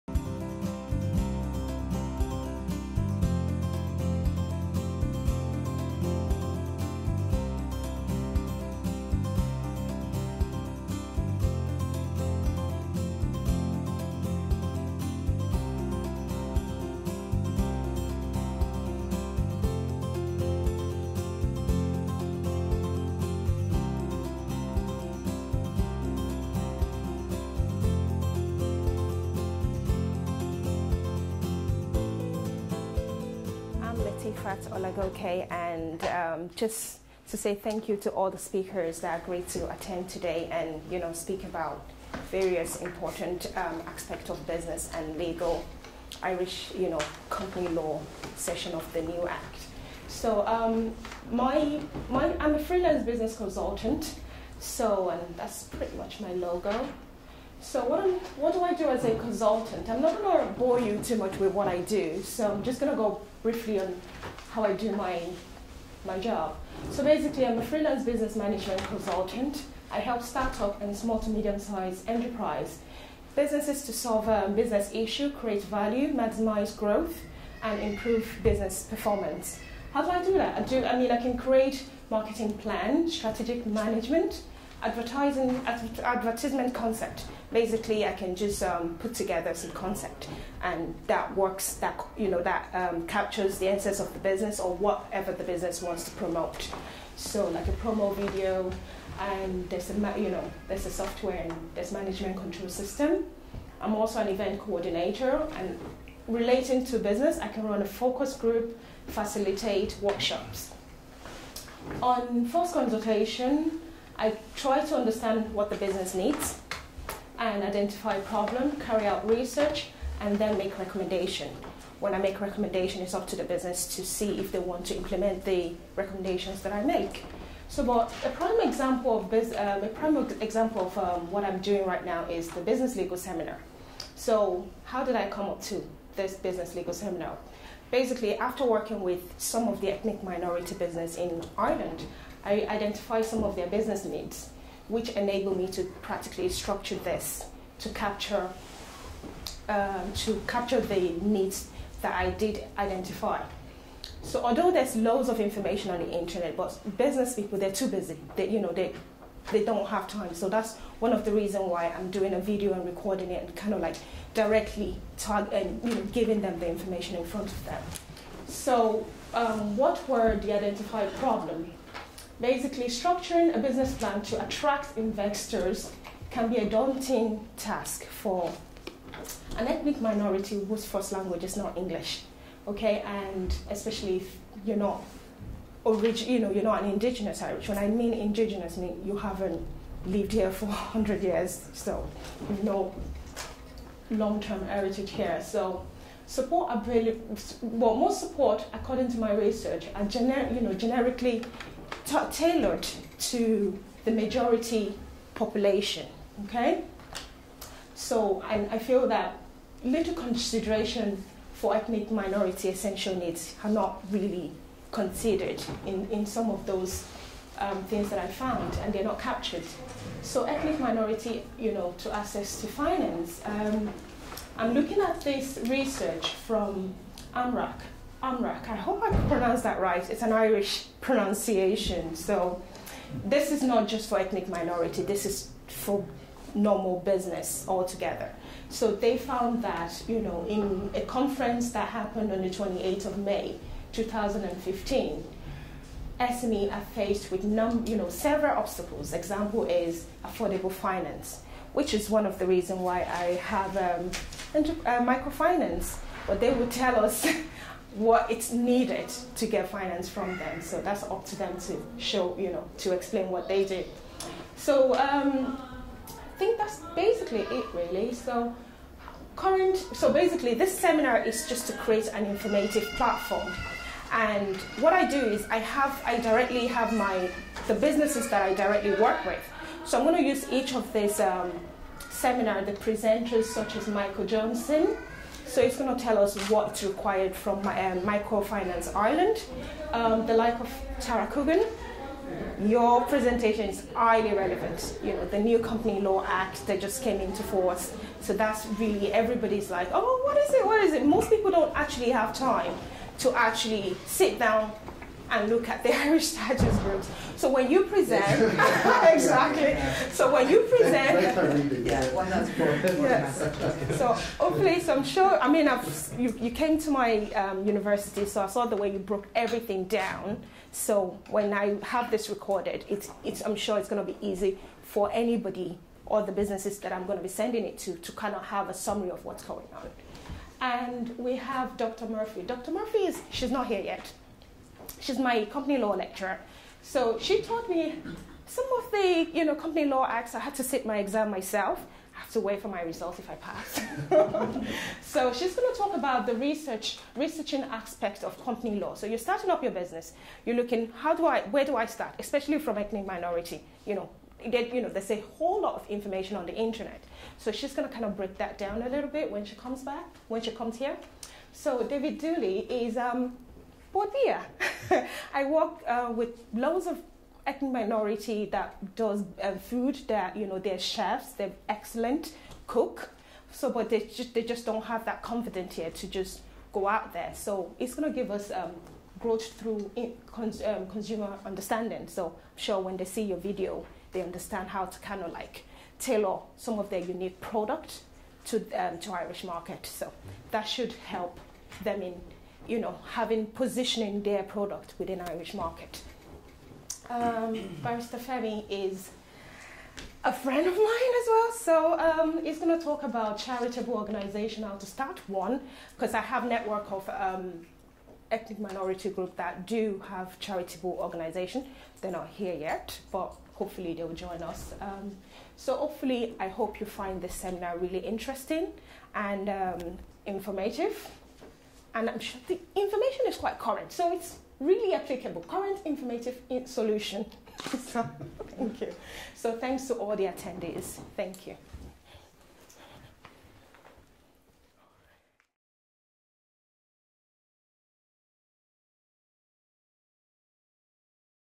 Business Legal Seminar- Latifat Consulting (6th of June 2015)
Business legal seminar was held on the 6th of June 2015- This was organised to raise further awareness of the new Company Act 2014 which commenced on the 1st of June 2015. By Latifat Consulting, guests speakers:

Michael Johnson: Topic “Ways in which to structure a business plan to attract investor- Requirement from Micro-Finance Ireland”

Tara Coogan:  Company Act 2014 Bill Vol. 1& 2

John O’Dea: Topic:  “Enterprise Ireland high potential start up and International focus business, funding / support”

Dr Clíodhna Murphy lecture in Company Law in Maynooth University. Topic: “DIY Company Law? Researching aspects of Company Law on the Internet”

David Deeley: Topic: “Growth Through Consumer Understanding”

Femi Daniyan: Topic: “Starting a Charitable Organisation and the New Charities Act of 2009”